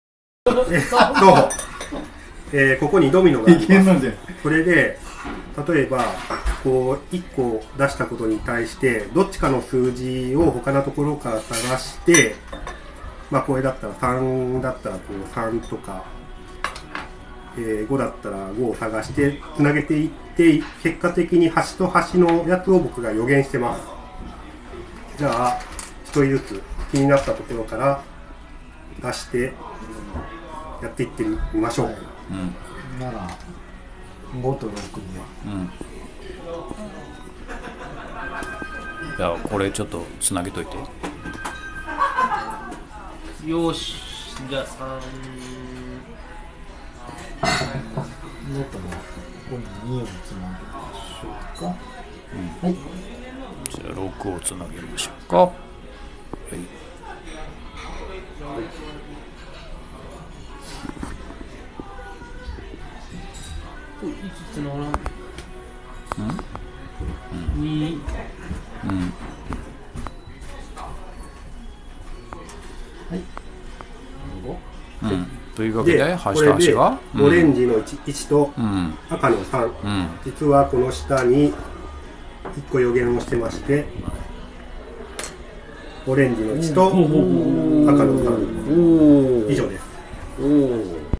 「手品・マジック・一発芸・超魔術」Mr.Qのマジックショー！テンヨー商品「ザ・ドミノ」です。
ドミノを適当に並べてもらい端と端との数値と色を予言するマジックです。